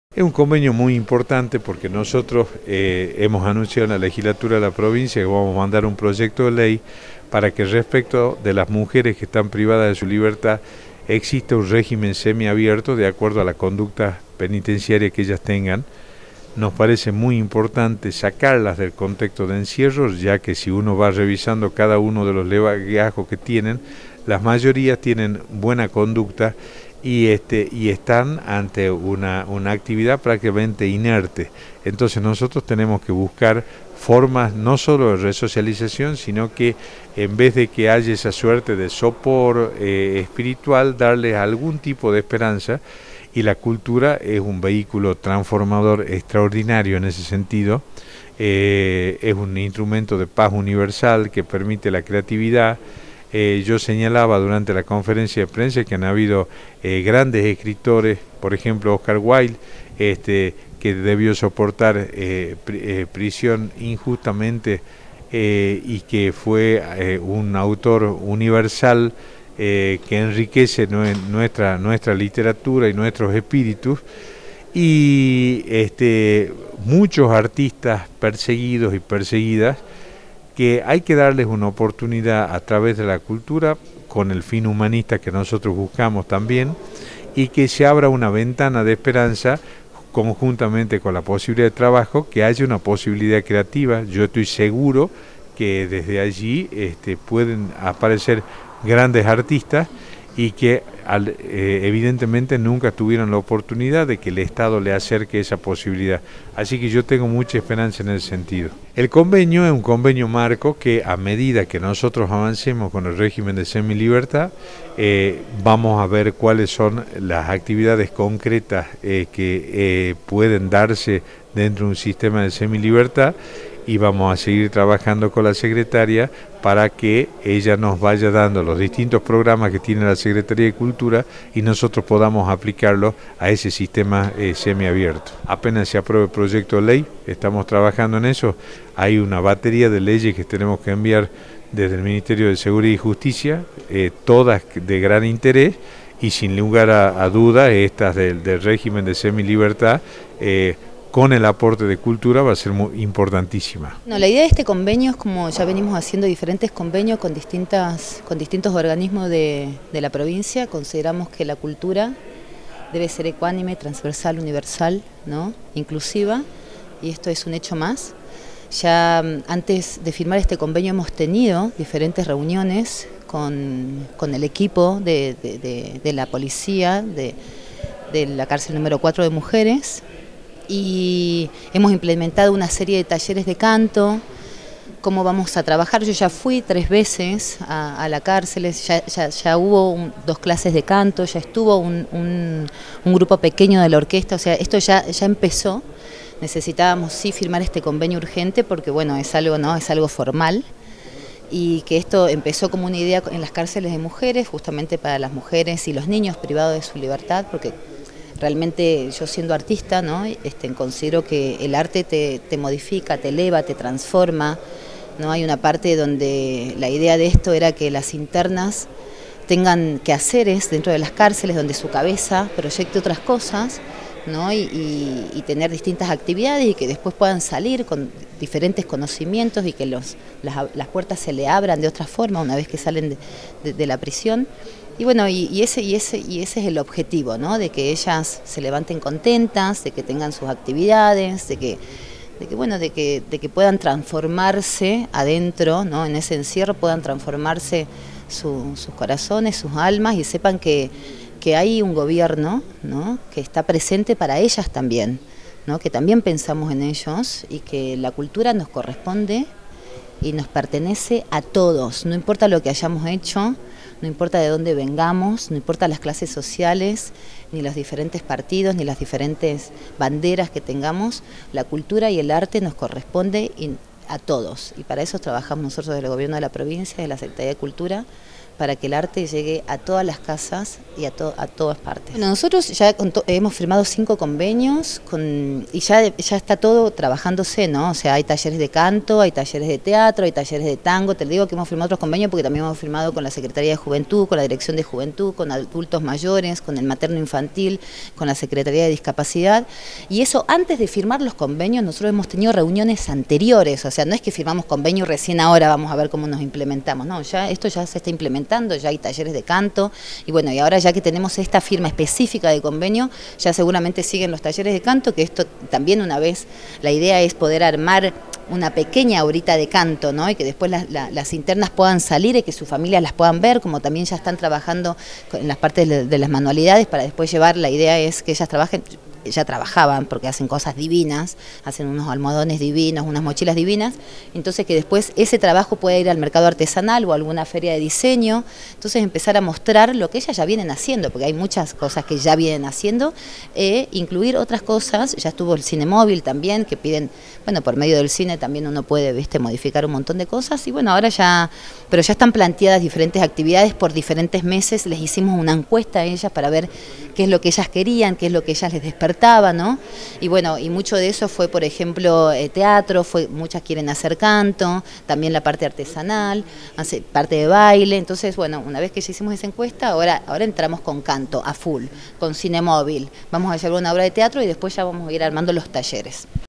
Profundizan las políticas de resocialización para mujeres privadas de la libertad
Nota al ministro de Seguridad, Abel Cornejo y a la secretaria de Cultura, Sabrina Sansone.